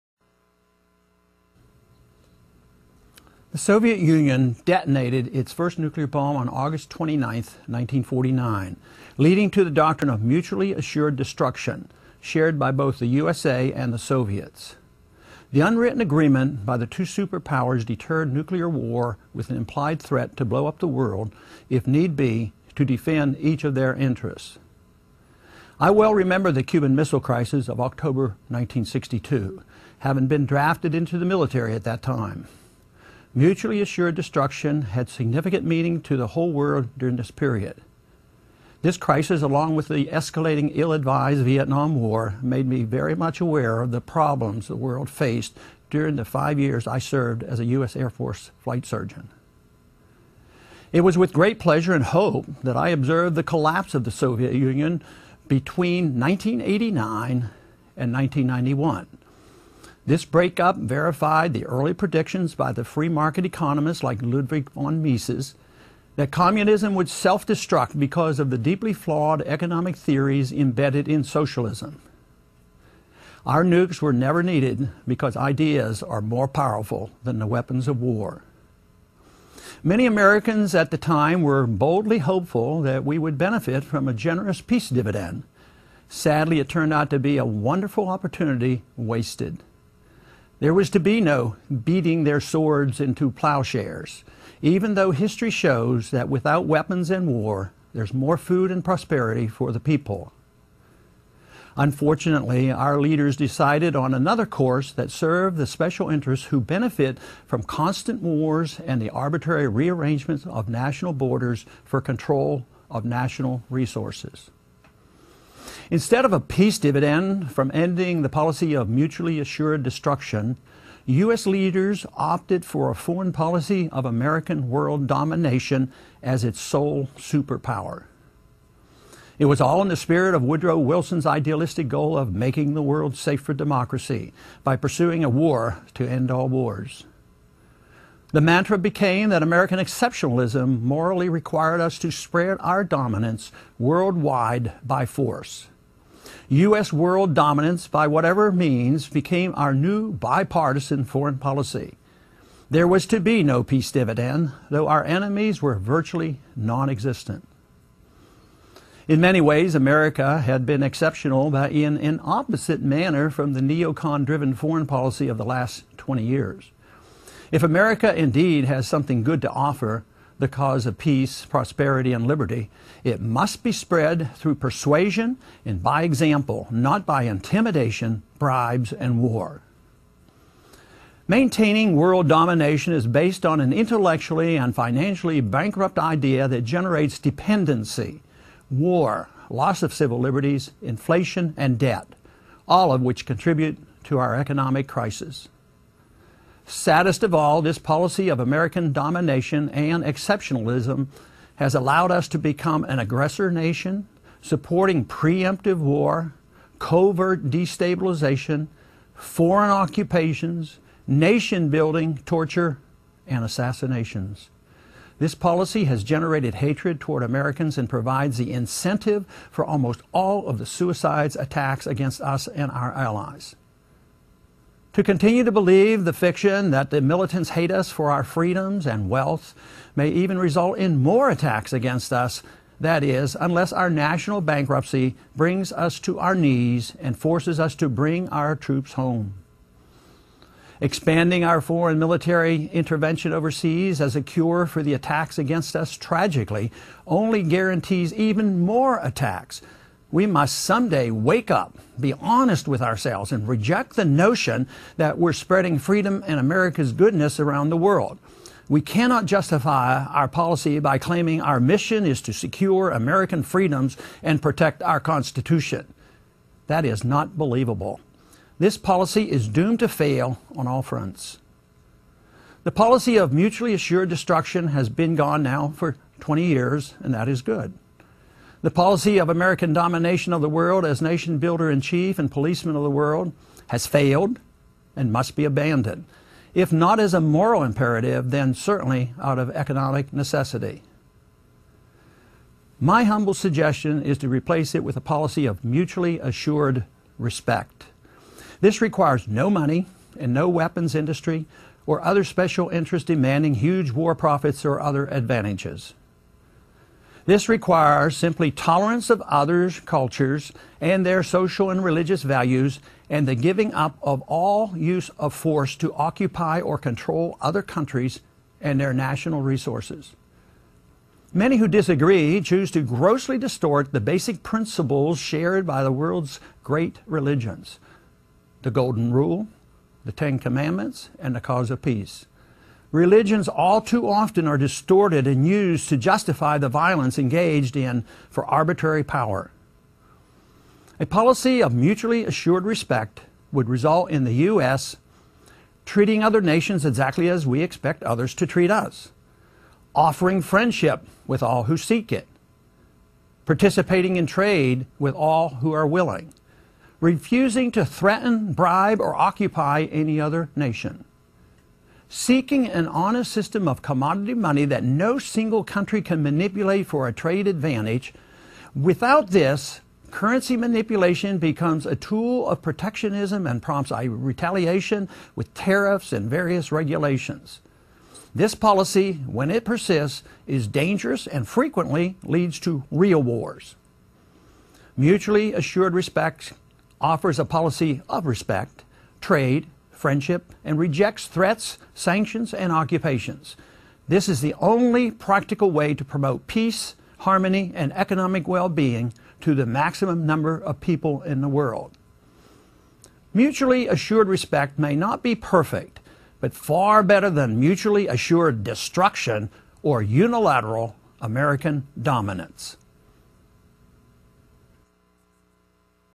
Ron Paul Mutually Assured Destruction vs Mutually Assured Respect 9/15/11
Mutually Assured Destruction vs Mutually Assured Respect - YouTube